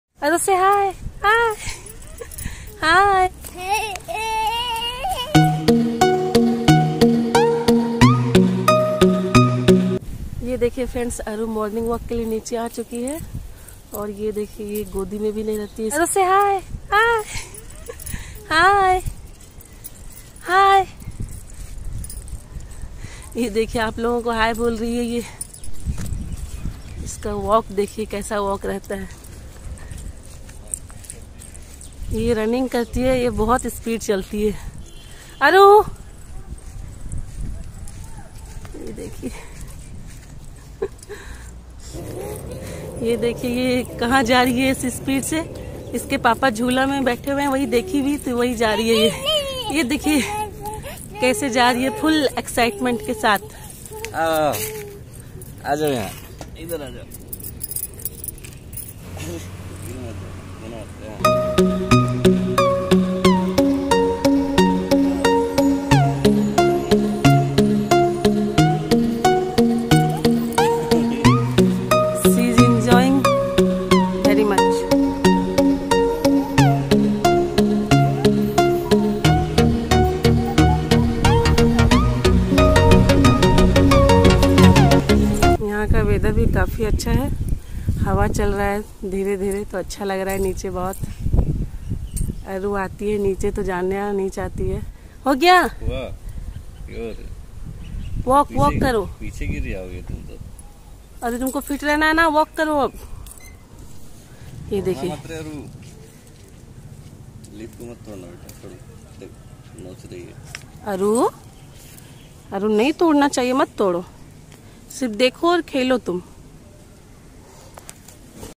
It's Morning walk OR Morning run for Aru ??? 🤔🤔🤔 | masti | greenery | society garden 🥰
It's Morning walk OR Morning run for Aru ??? 🤔🤔🤔🤔🤔 |
in this video my 1 year old daughter is doing morning walk
Aru doing morning walk
are you doing morning walk at garden
aru enjoyment  in park🥰👌 | nature | greenery | masti | kolkata
in this video my baby is playing in park